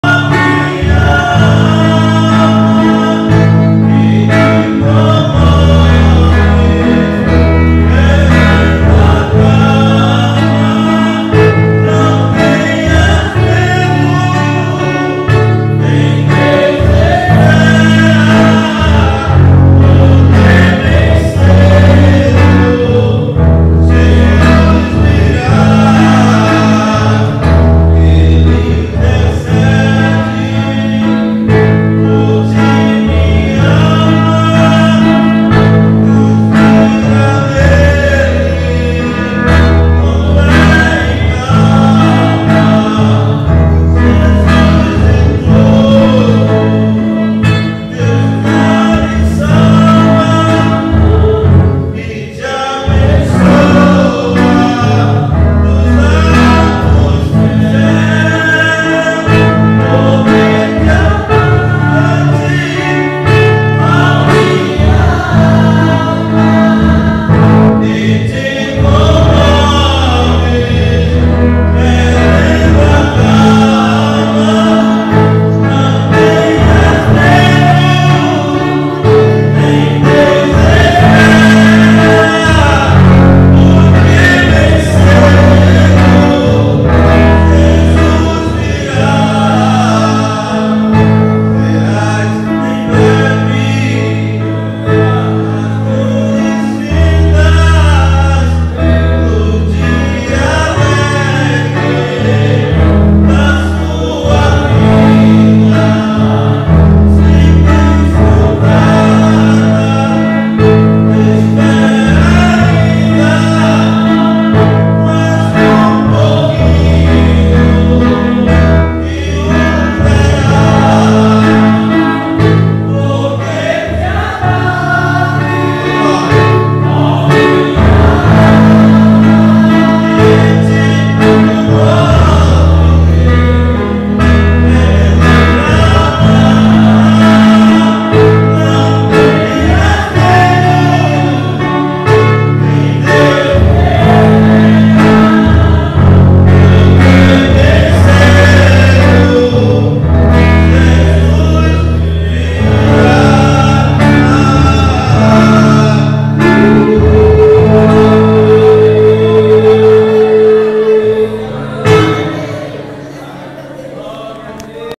Última apresentação de Jor Santana. Crédito:Alfredo Neto/Blog do Anderson Pereira
Última apresentação do cantor Jor Santana, na 1ª Igreja Batista, em Goiana, Zona da Mata Norte de Pernambuco.